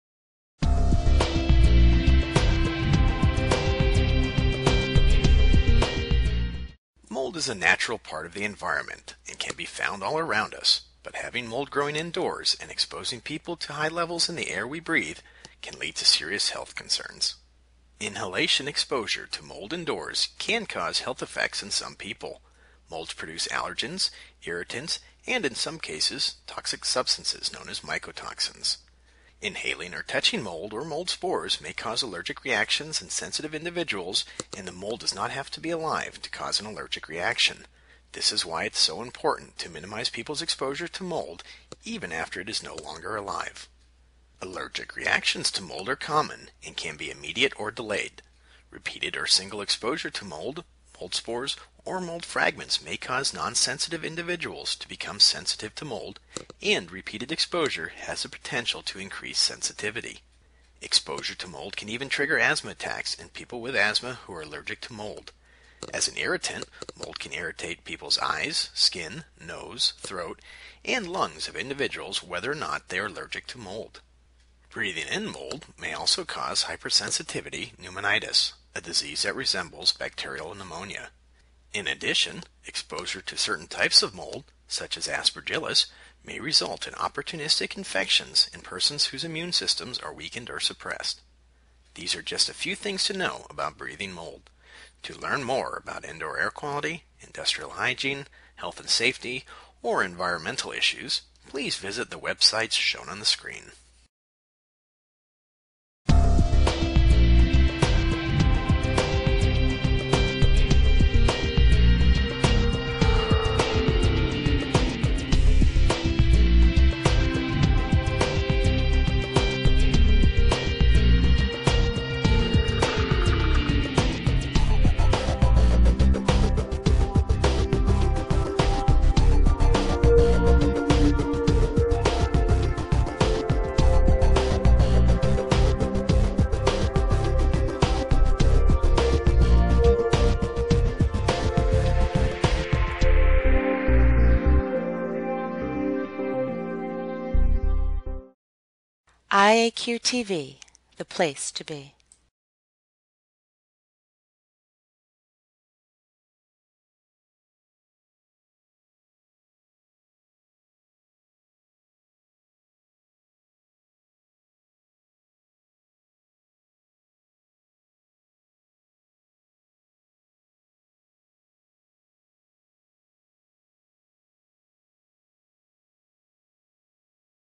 Breathing Mold Can Cause Health Issues
Mold is a natural part of the environment and can be found all around us, but having mold growing indoors and exposing people to high levels in the air we breathe can lead to serious health concerns.

Inhalation exposure to mold indoors can cause health effects in some people. Molds produce allergens, irritants, and, in some cases, toxic substances known as mycotoxins.

Inhaling or touching mold or mold spores may cause allergic reactions in sensitive individuals and the mold does not have to be alive to cause an allergic reaction. This is why it is important to minimize people's exposure to mold, even after it is no longer alive.

Allergic reactions to mold are common and can be immediate or delayed. Repeated or single exposure to mold, mold spores, or mold fragments may cause non-sensitive individuals to become sensitive to mold, and repeated exposure has the potential to increase sensitivity.

Exposure to mold can even trigger asthma attacks in people with asthma who are allergic to mold. As an irritant, mold can irritate people's eyes, skin, nose, throat, and lungs of individuals whether or not they are allergic to mold.

Breathing in mold may also cause hypersensitivity pneumonitis, a disease that resembles bacterial pneumonia. In addition, exposure to certain types of mold, such as Aspergillus, may result in opportunistic infections in persons whose immune systems are weakened or suppressed.